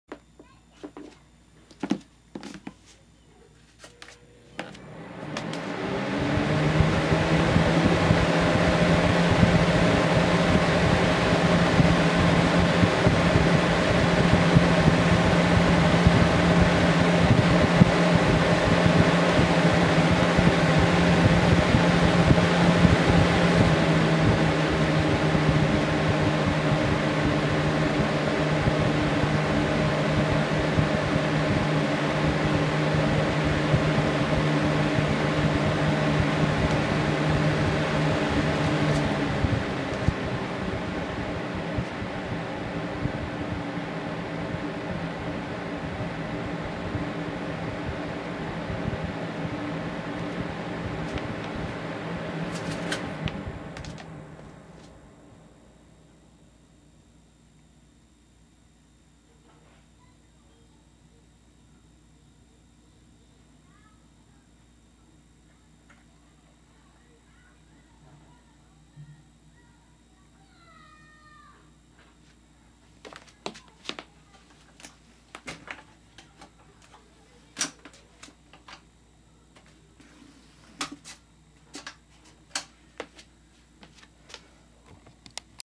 2008 Galaxy box fan
Personally I'm surprised this fan still works, its had a fair amount of use over the years.  Even though I service it, I have to wonder how much longer it will last.